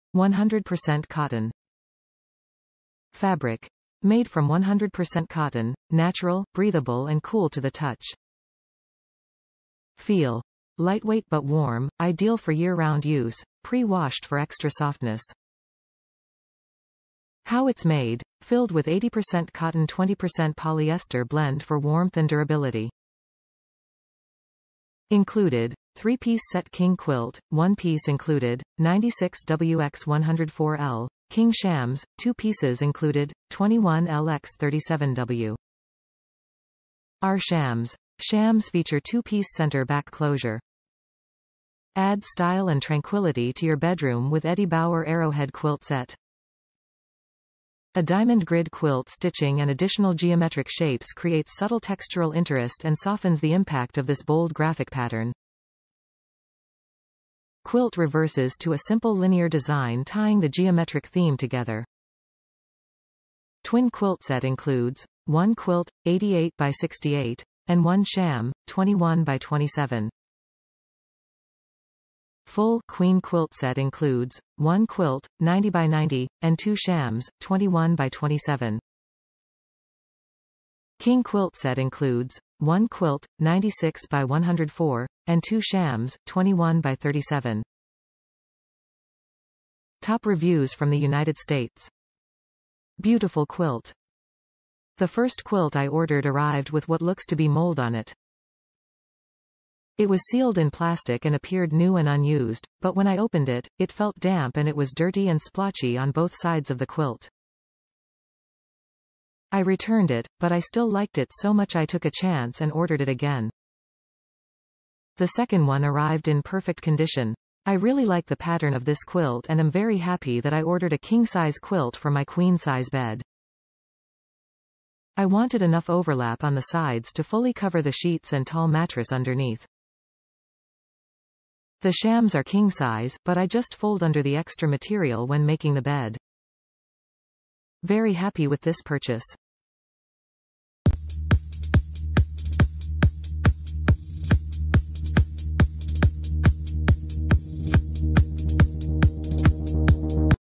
Eddie Bauer - King Quilt Set, Reversible Cotton Bedding with Matching Shams
100% Cotton
FABRIC: Made from 100% Cotton; natural, breathable and cool to the touch
FEEL: Lightweight but warm, ideal for year-round use; Pre-washed for extra softness
HOW ITS MADE: Filled with 80% Cotton 20% Polyester blend for warmth and durability
INCLUDED: 3 PIECE SET- King Quilt (1-Piece Included): 96"W X 104"L, King Shams (2-Pieces Included): 21"L X 37"W
OUR SHAMS: Shams feature 2-piece center back closure
Add style and tranquility to your bedroom with Eddie Bauer arrowhead quilt set. A diamond grid quilt stitching and additional geometric shapes creates subtle textural interest and softens the impact of this bold graphic pattern. Quilt reverses to a simple linear design tying the geometric theme together. Twin quilt set includes: one quilt (88"x68") and one sham (21"x27"). full/queen quilt set includes: one quilt (90"x90") and two shams (21"x27"). King quilt set includes: one quilt (96"x104") and two shams (21"x37").

Beautiful quilt!
The first quilt I ordered arrived with what looks to be mold on it. It was sealed in plastic and appeared new and unused, but when I opened it, it felt damp and it was dirty and splotchy on both sides of the quilt. I returned it, but I still liked it so much I took a chance and ordered it again. The second one arrived in perfect condition! I really like the pattern of this quilt and am very happy that I ordered a king size quilt for my queen size bed. I wanted enough overlap on the sides to fully cover the sheets and tall mattress underneath. The shams are king-size, but I just fold under the extra material when making the bed. Very happy with this purchase!